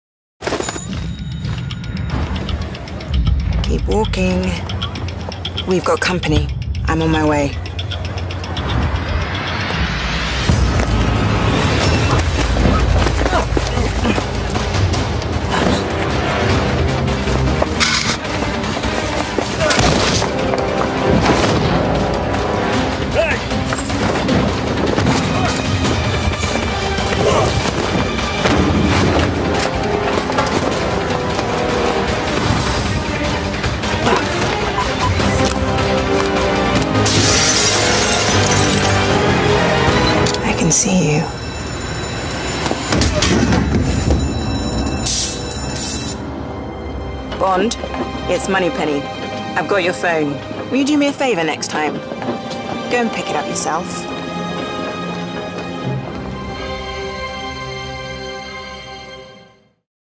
Made for Bond. Sony Xperia phones available
“Made for Bond” Clip features Moneypenny(Naomie Harris) from James Bond movies - SkyFall and Spectre.